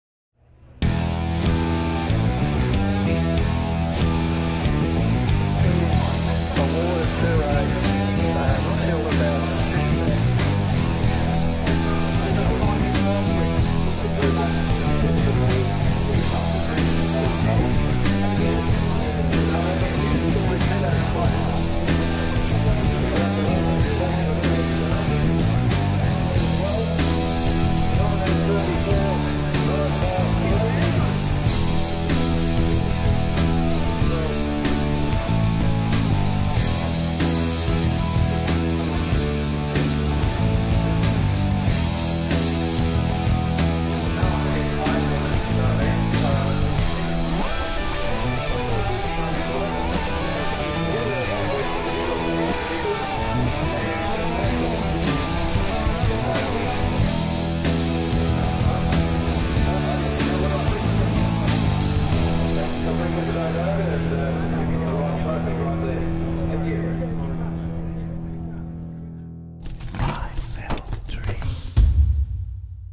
Healey "4000" -- The Healey Factory
Check out The Healey Factory display from the Classic Showcase 2012, showcasing the rare Healey "4000" -- one of the three cars ever produced and the only one personally completed by Donald Healey.